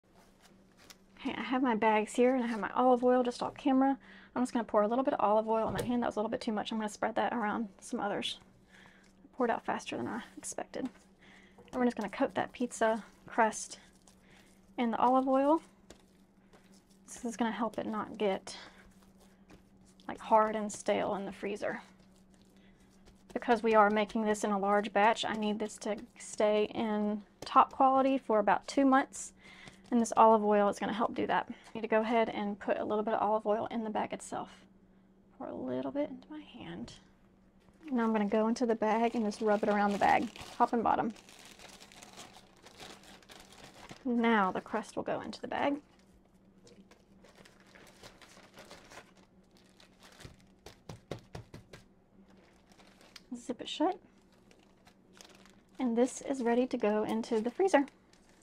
How to Freeze Pizza Dough
Here's how to freeze your dough for future use without the dough getting stale, freezer-burned, or stuck to the bag.